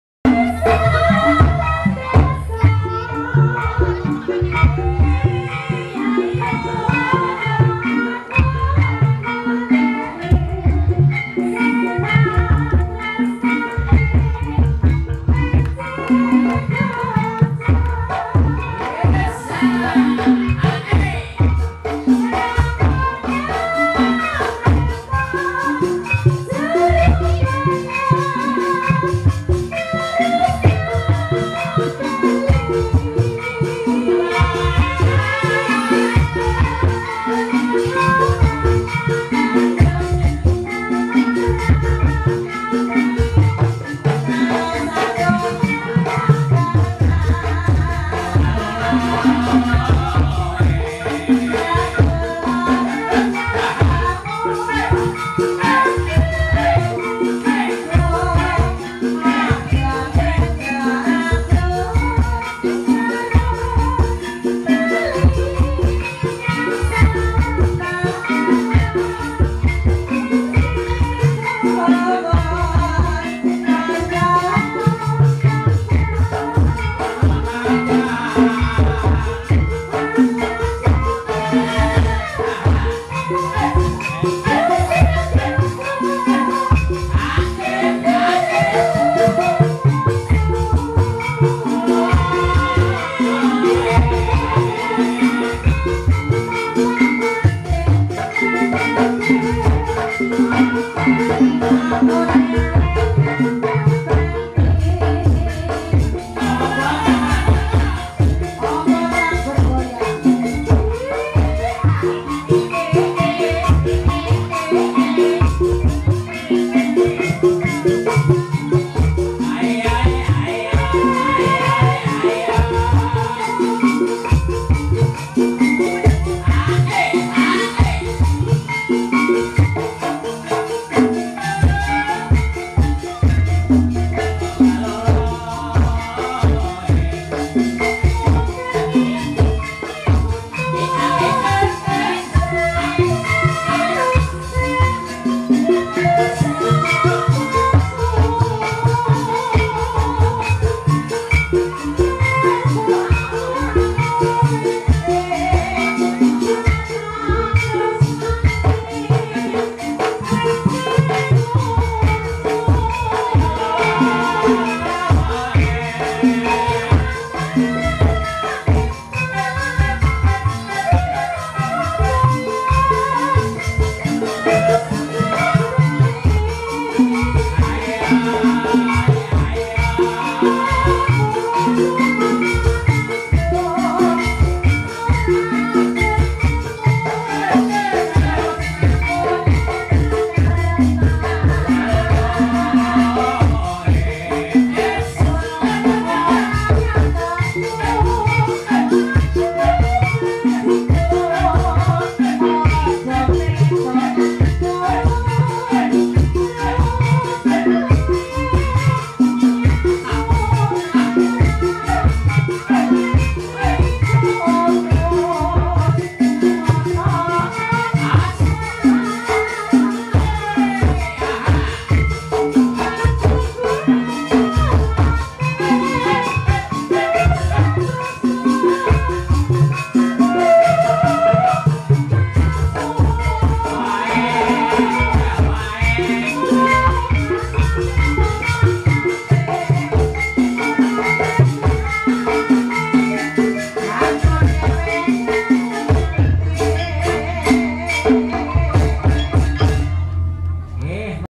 Kelinci Ucul Cepot Ebeg Wijaya Sari di Pengadegan Wangon
26 Desember 2020 Petis Manis Ebeg Wijaya Sari pentas di Pengadegan Wangon Banyumas